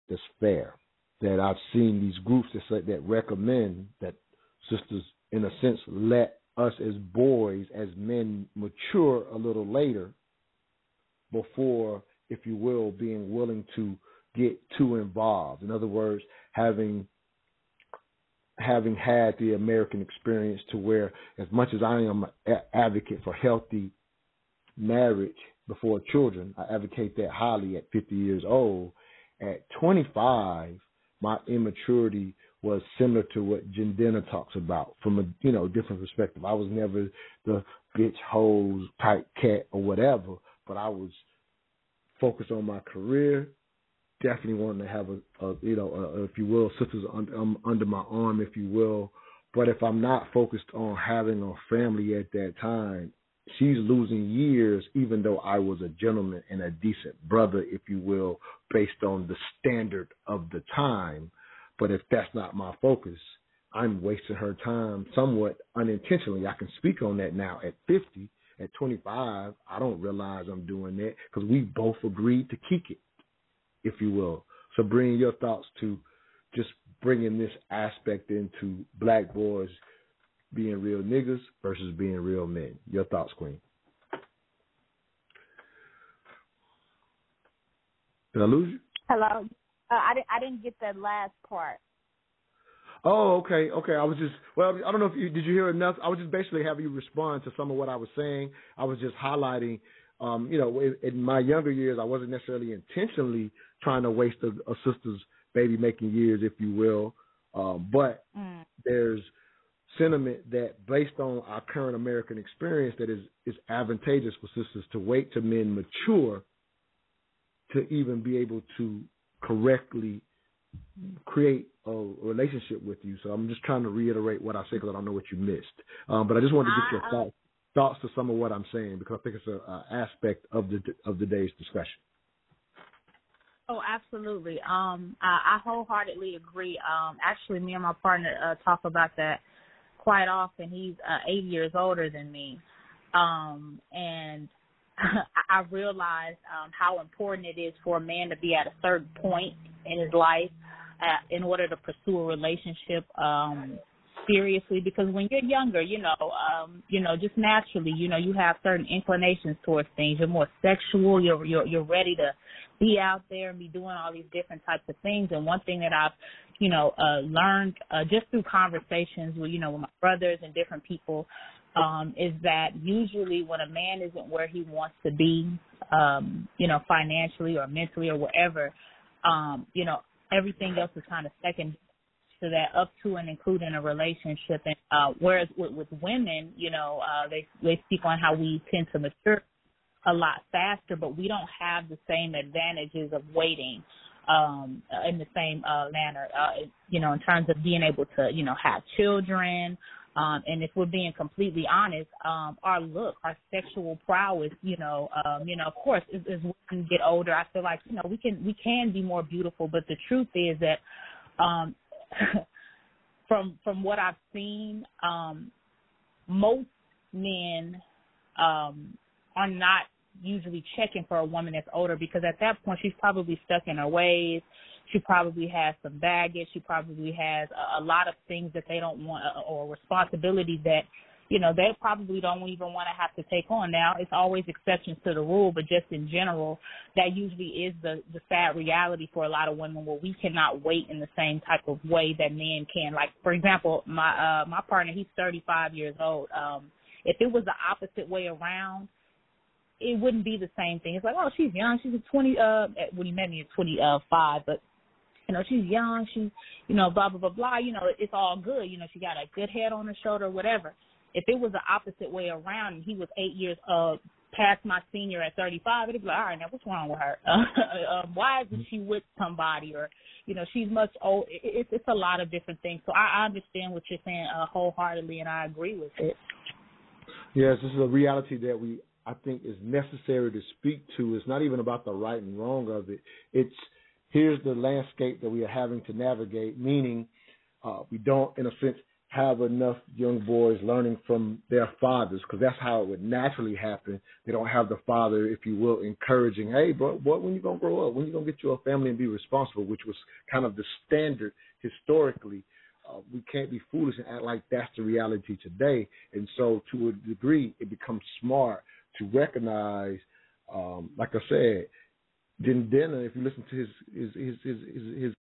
Do Black Men waste Black women's time during their best baby-making years?
"Does Black Culture Raise Boys To Be Real Niggaz Or Real Men?"